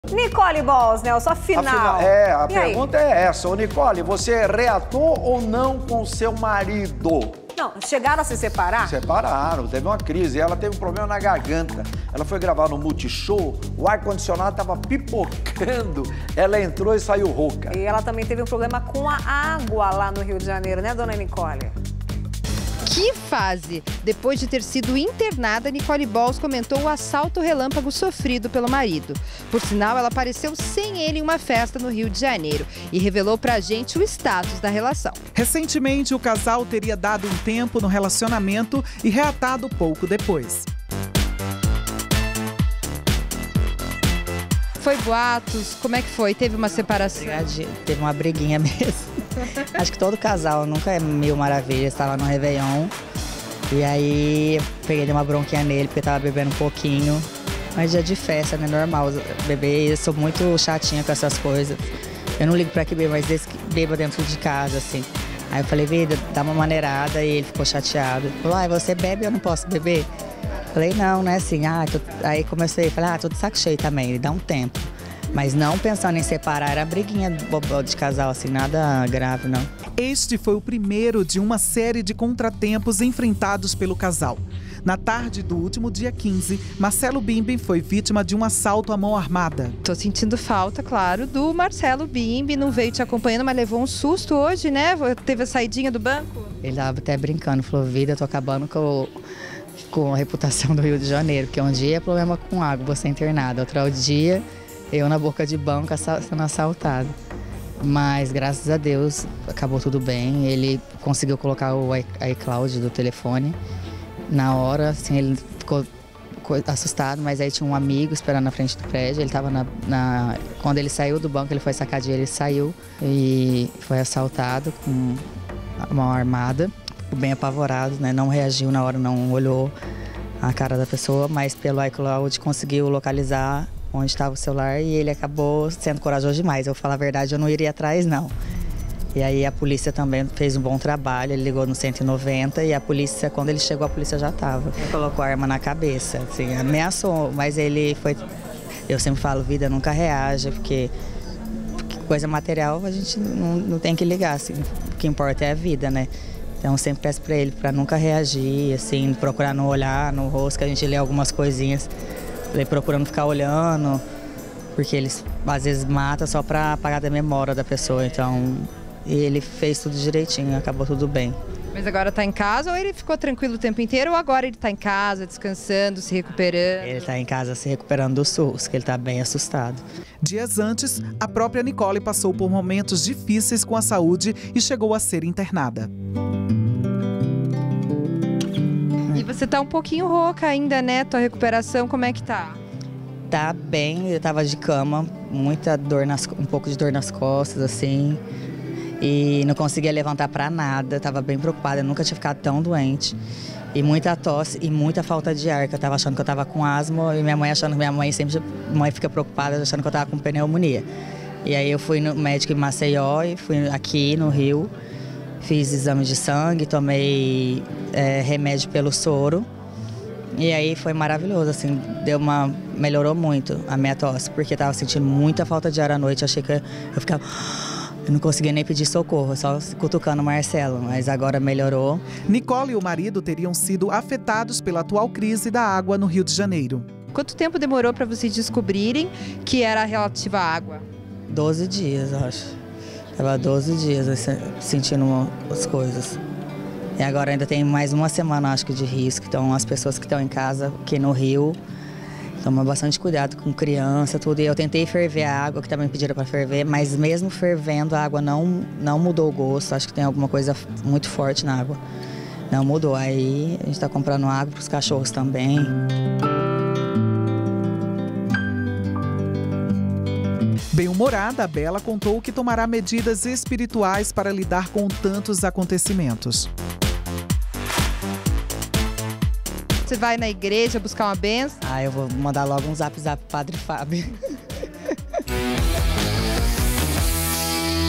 Nicole Bahls diz que ficou internada após ingerir água contaminada no RJ
A modelo Nicole Bahls ficou hospitalizada após adoecer ao ingerir água encanada no Rio de Janeiro. Ao TV FAMA, ela conta que passou mal com uma crise respiratória.

"Eu fiquei de cama, muita dor nas costas, não conseguia levantar pra nada. Nunca fiquei tão doente. Eu fui no médico, fiz exames, fui medicada e melhorou bastante. Eu tinha muita falta de ar, não conseguia nem pedir socorro", disse. "Doze dias sentindo as coisas. Agora tem mais uma semana de risco. Tomem bastante cuidado. Eu tentei ferver, mas mesmo assim não mudou o gosto. A gente tá comprando água", afirmou Bahls.

Questionada sobre uma crise no relacionamento com Marcelo Bimbi, Nicole explica que só foi uma "briguinha besta". "Nunca é as mil maravilhas. Estava no ano novo, dei uma bronquinha porque ele estava bebendo muito. Pedi pra dar uma maneirada e ele ficou chateado. Pedi um tempo, mas não era pensando em se separar, era briguinha de casal, nada grave", esclareceu ela.